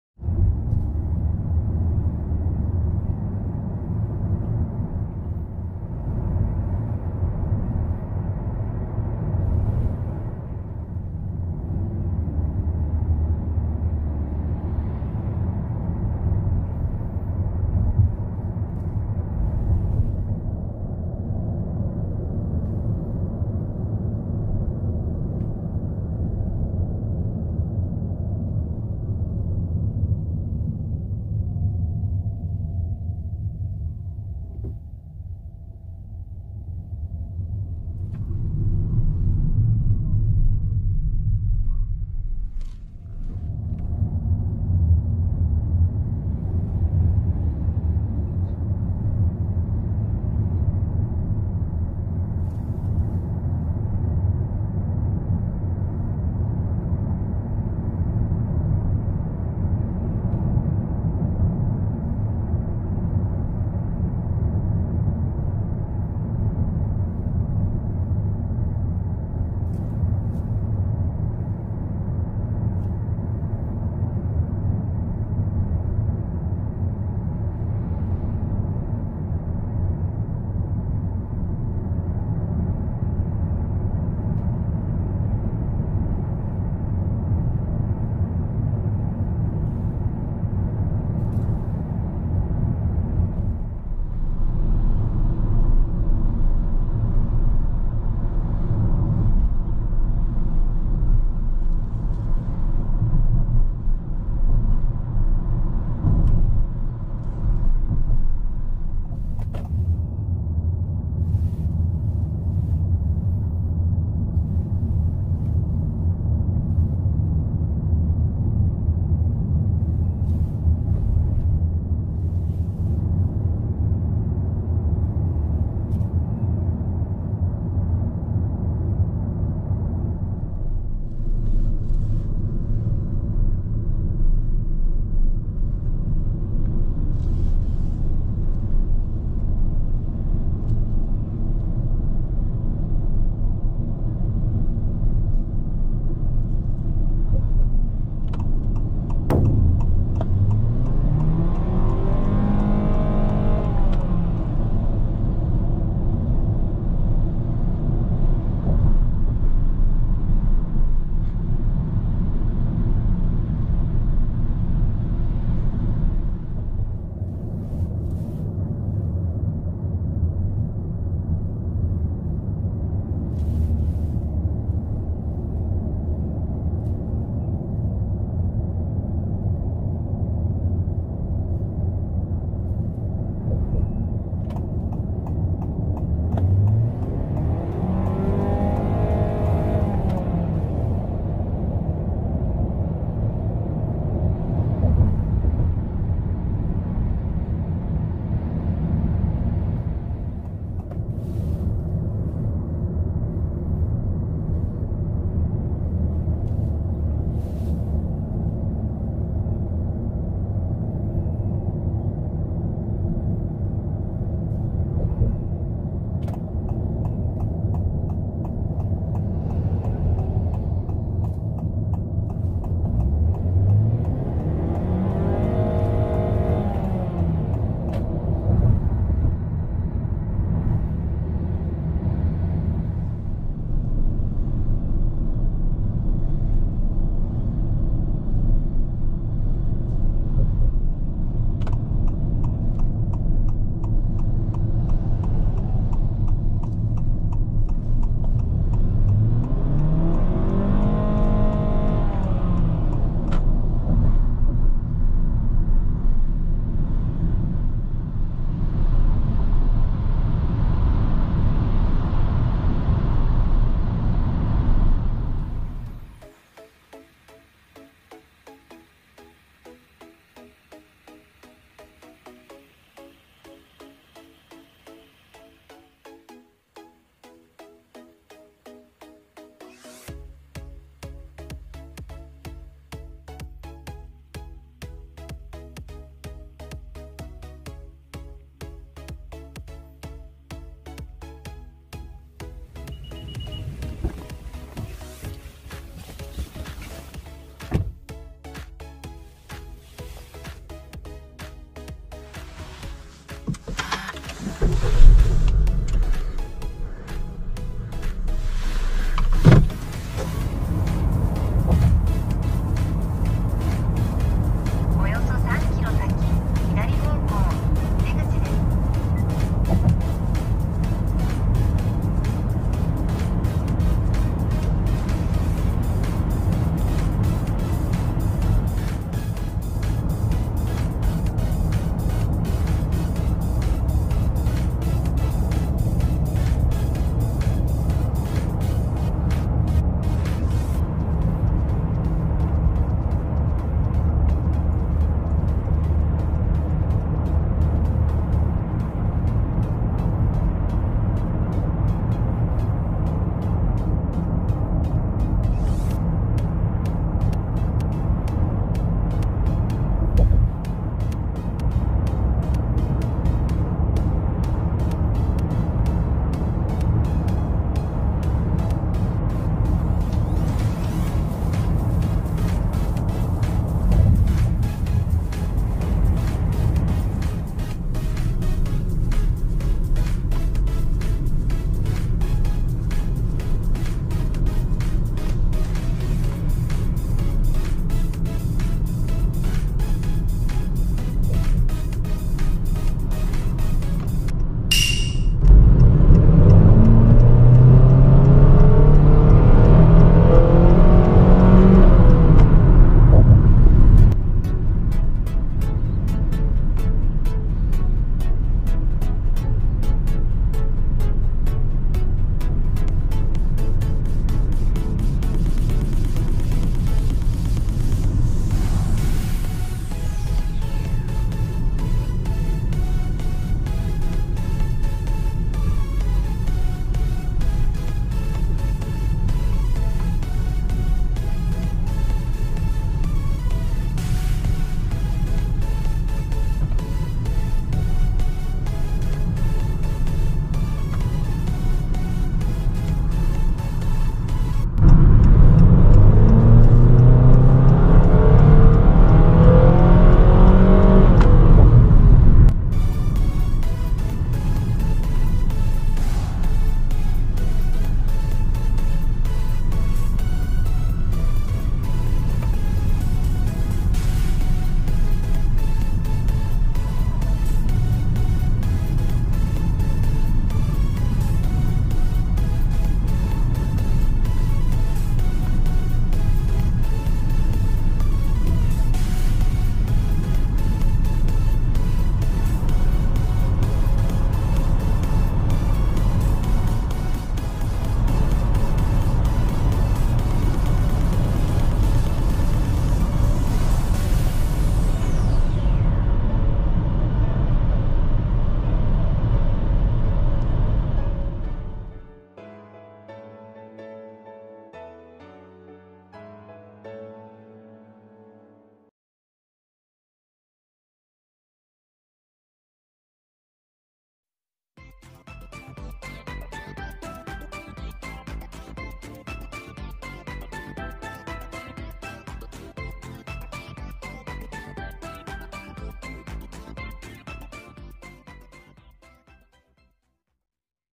新型ステップワゴン vs 新型オデッセイ！ベタ踏みして加速力を比較した結果…！ホンダ ミニバン 新型スパーダ
➡︎ホンダの人気ミニバン「ステップワゴンエアー」「オデッセイ」
合流地点前の加速車線でベタ踏みして加速力を比較してみた。